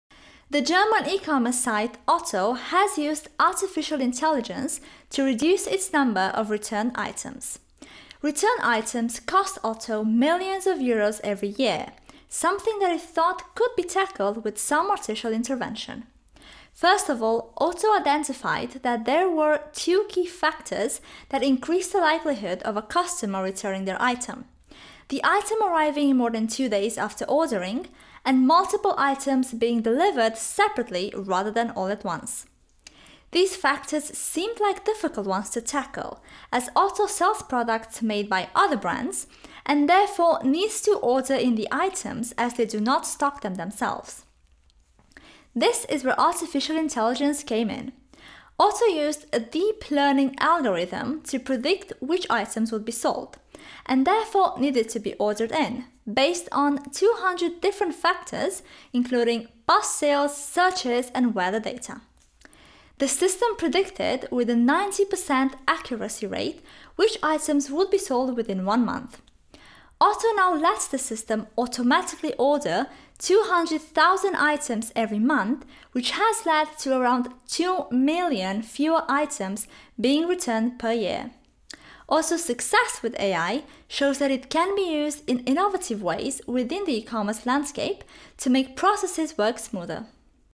Otto has used AI to reduce its returns
The German ecommerce site Otto has used artificial intelligence to reduce its number of returned items.

Returned items cost Otto millions of Euros every year, something that it thought could be tackled with some artificial intervention.

First of all, Otto identified that there were two key factors that increased the likelihood of a customer returning their item: the item arriving more than two days after ordering, and multiple items being delivered separately rather than all at once.

These factors seemed like difficult ones to tackle, as Otto sells products made by other brands and therefore needs to order in the items as they do not stock them themselves.

This is where artificial intelligence came in. Otto used a deep learning algorithm to predict which items would be sold – and therefore needed to be ordered in – based on 200 different factors including past sales, searches and weather data.

The system predicted, with a 90% accuracy rate, which items would be sold within a one month period. Otto now lets the system automatically order 200,000 items every month, which has led to around 2 million fewer items being returned per year.

Otto’s success with AI shows that it can be used in innovative ways within the ecommerce landscape to make processes work smoother.